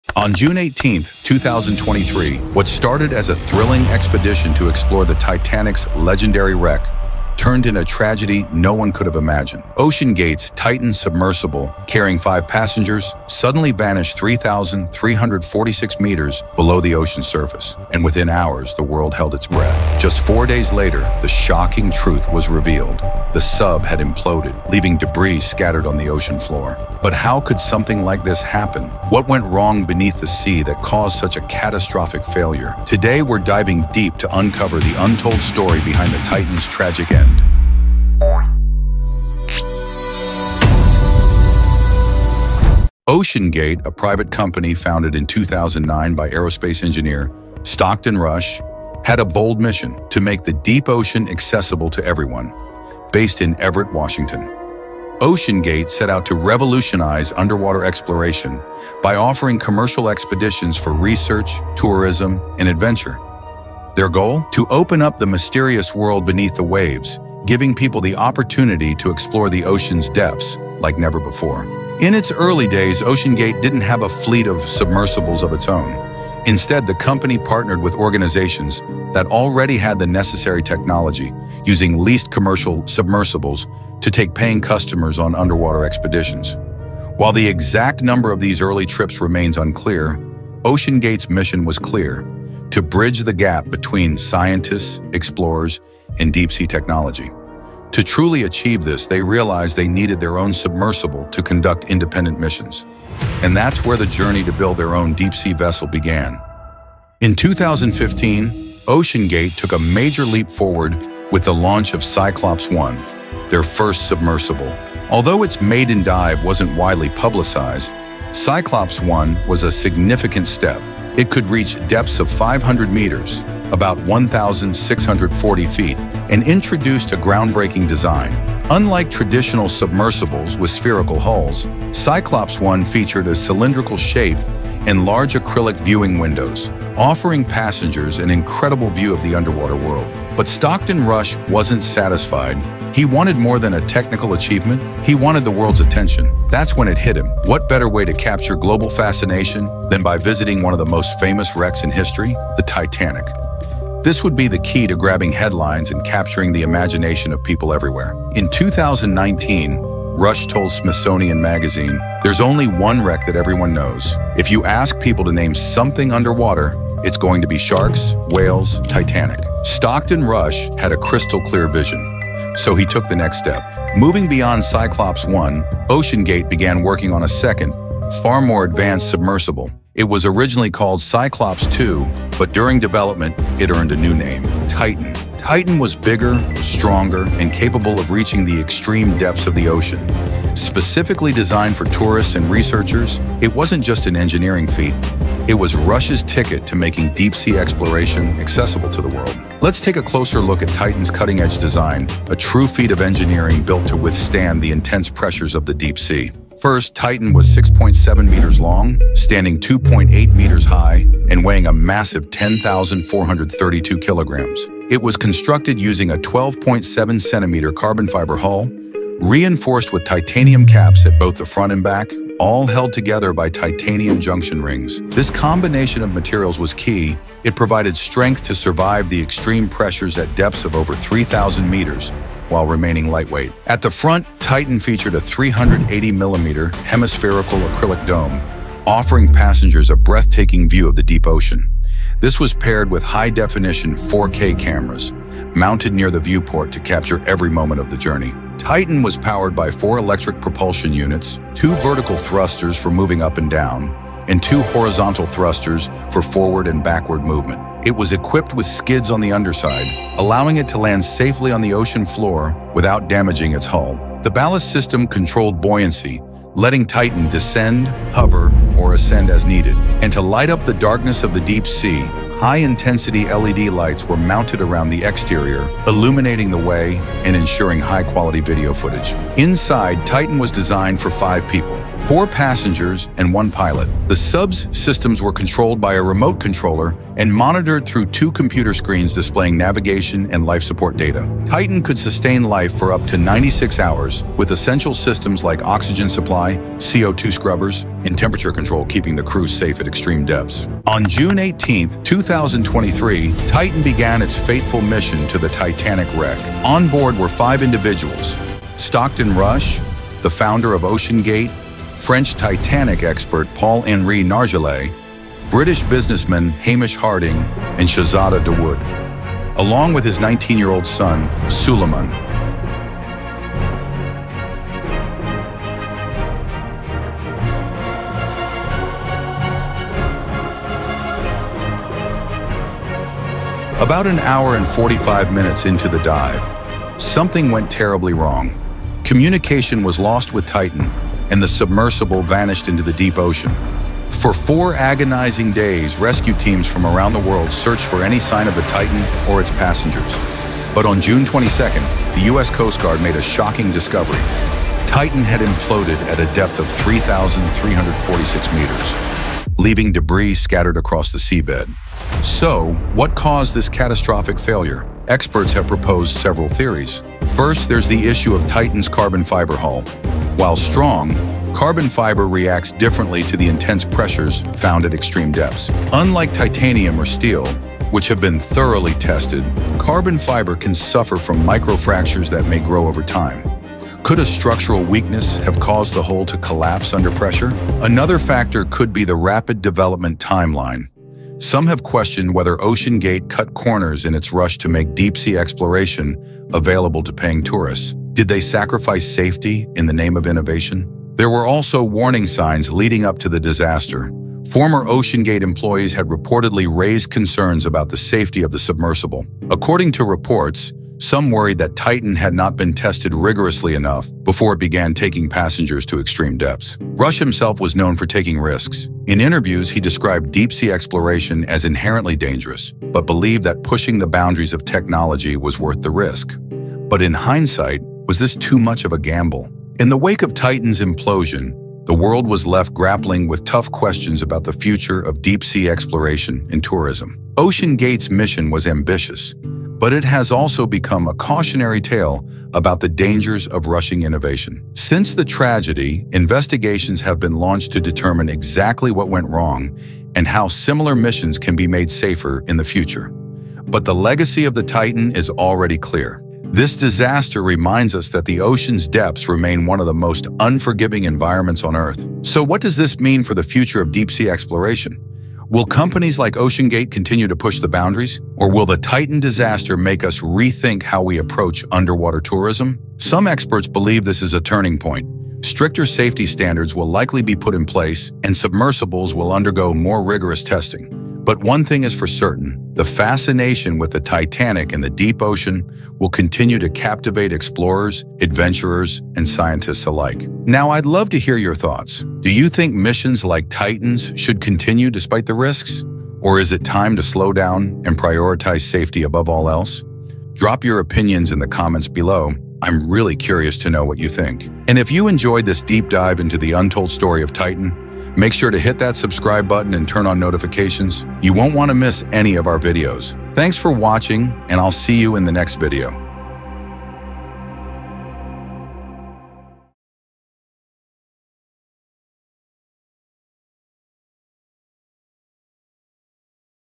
Untold Fatal Story of Titan Submersible | What Went Wrong?
In this video, we uncover what really happened during OceanGate’s Titan Submersible tragedy. Delve into the details of the Titan submersible implosion that shocked the world, as we explore the events leading up to the OceanGate disaster. The ill-fated Titanic wreckage expedition was meant to push the boundaries of deep-sea exploration, but the risks became all too real. Discover how the carbon fiber submersible failure played a key role in this tragic outcome and learn about the lessons drawn from OceanGate’s Titan tragedy.